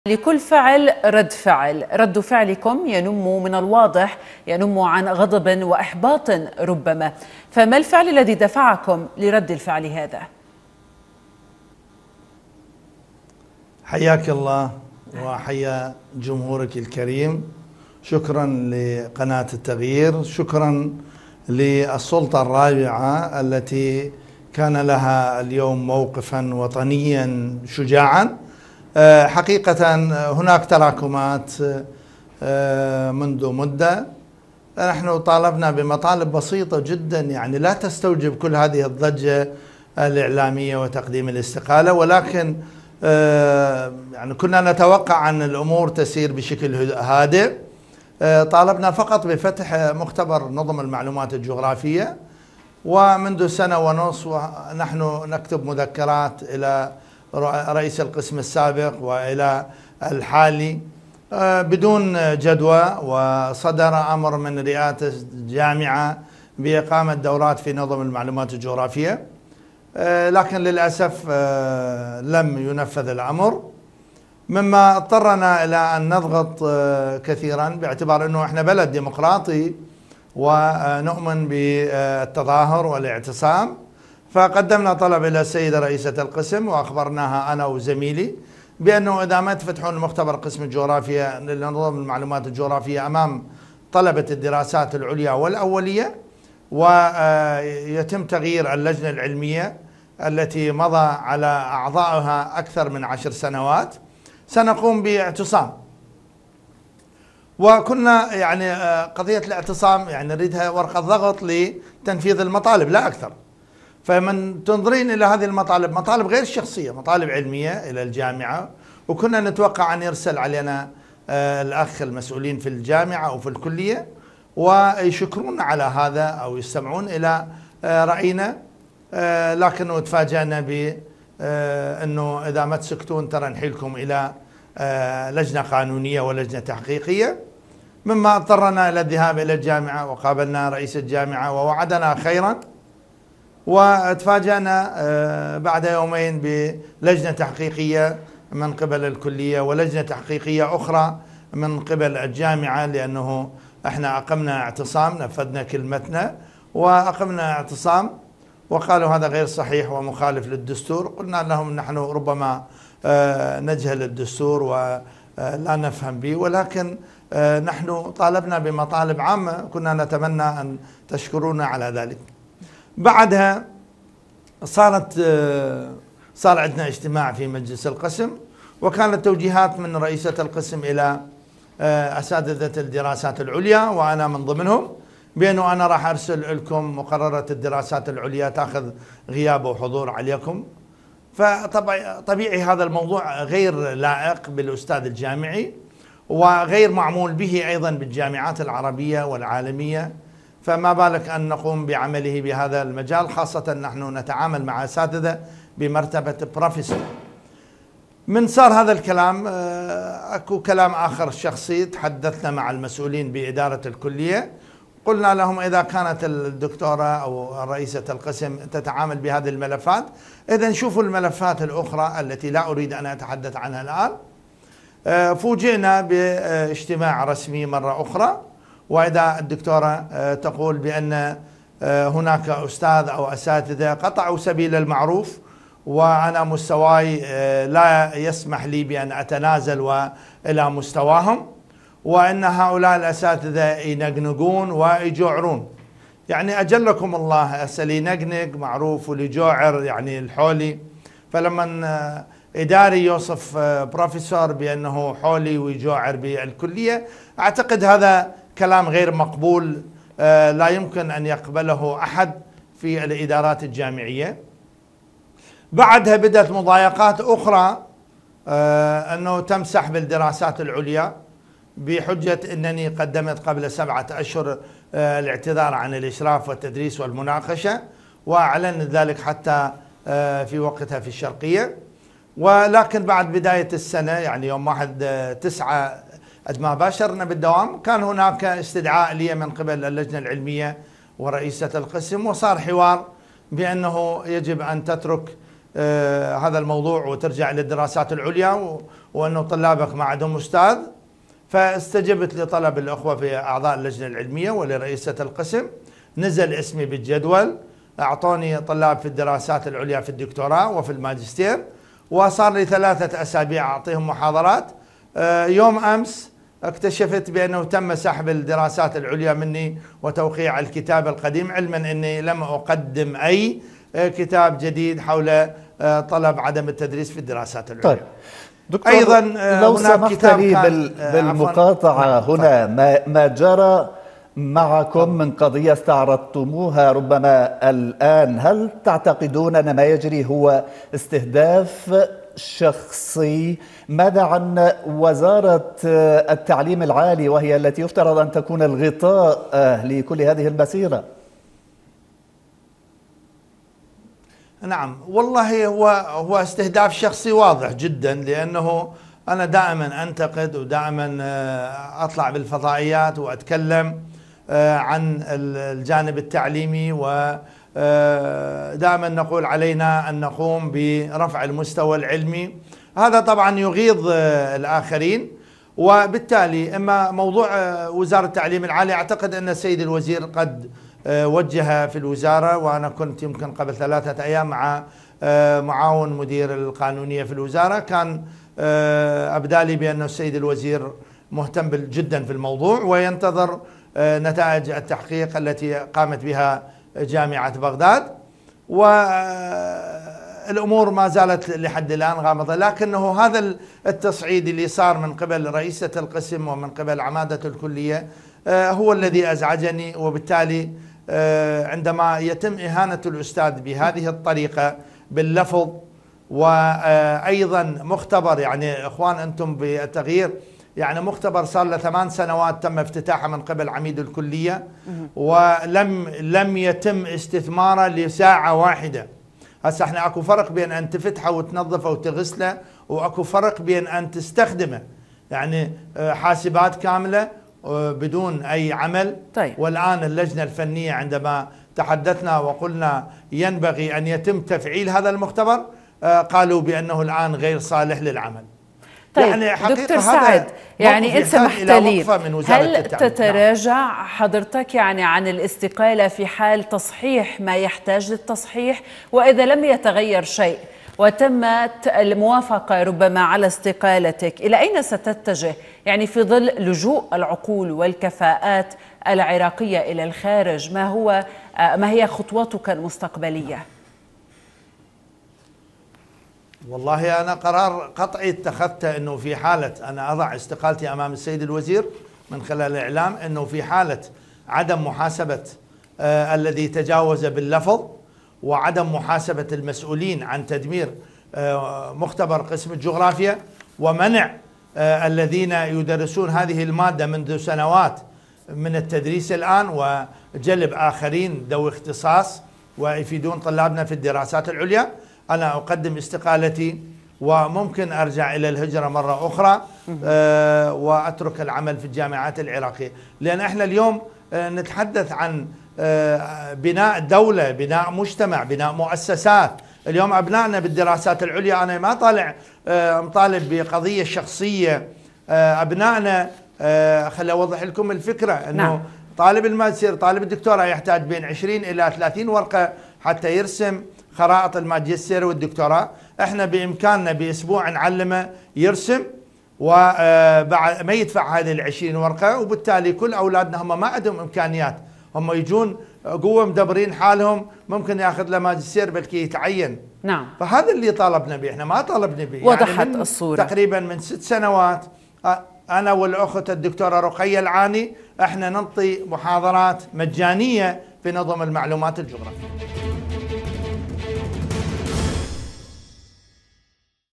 بروفيسور عراقي يكشف اسباب حرقه شهاداته ومؤلفاته
موقع قناة التغيير الفضائية الالكتروني